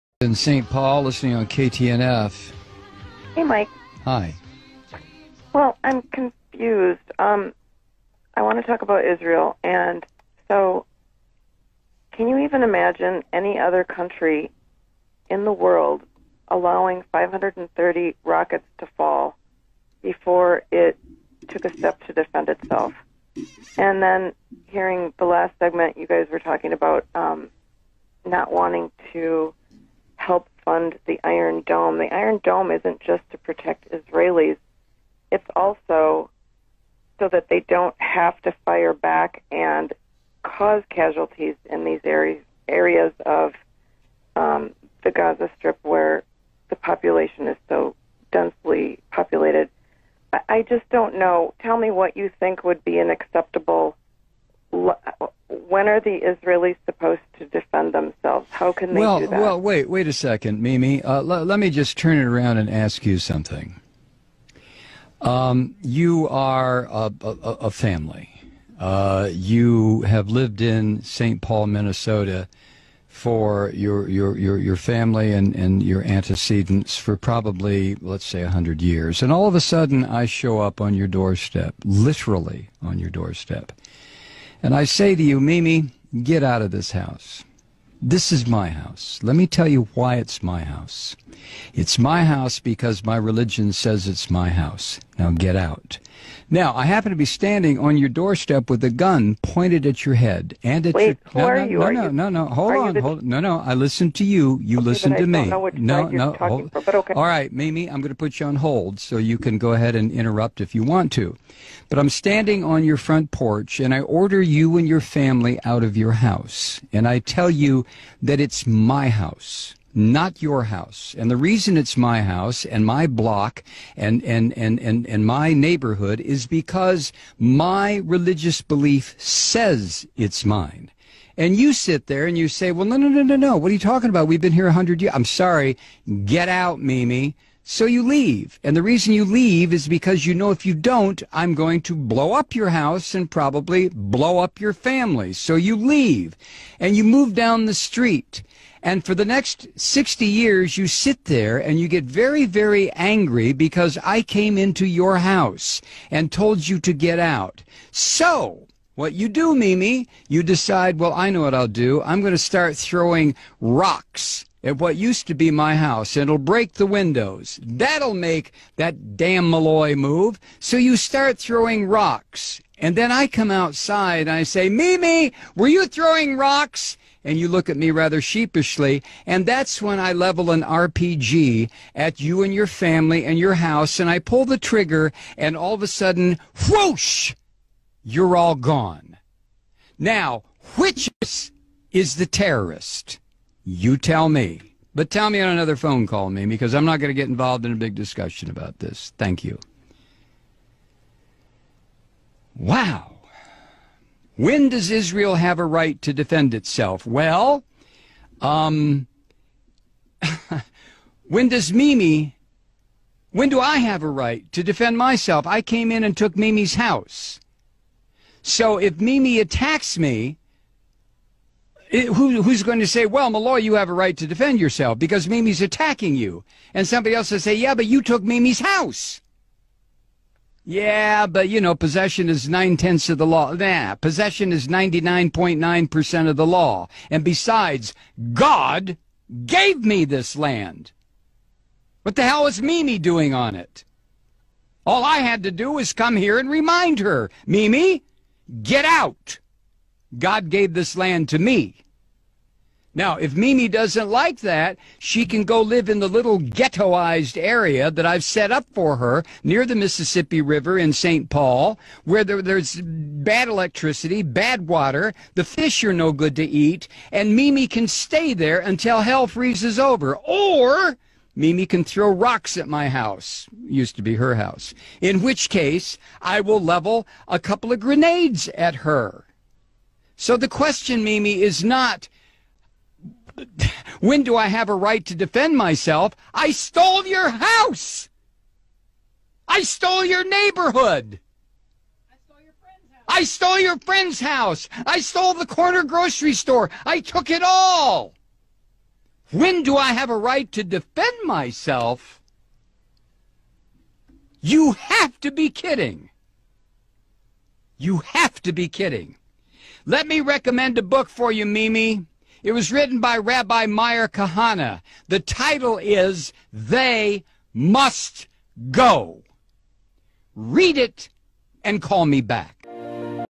Juruhebah AS, Mike Malloy Membela Hak Rakyat Palestin!
Negara haram Israel membunuh wanita dan kanak² tanpa pembelaan sewajarnya dari dunia.

Semoga mereka diberi kemenangan menentang kekejaman pengganas zionis. Amiin